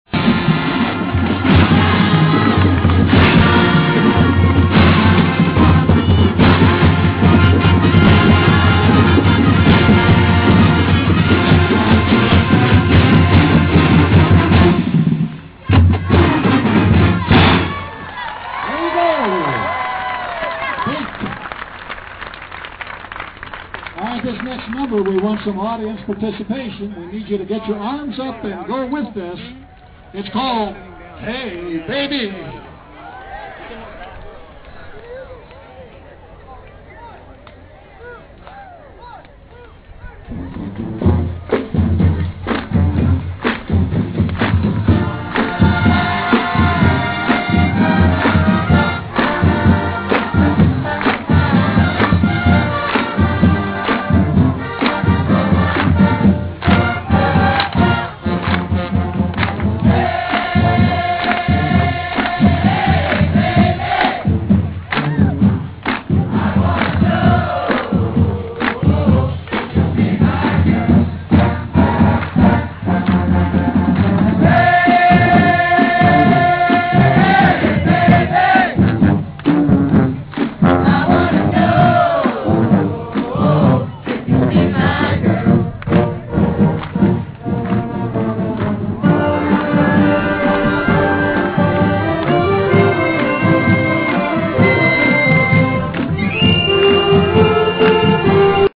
Iowa Hawkeye cheerleaders and band on the beach in Tampa
Video by Jonathan D. Woods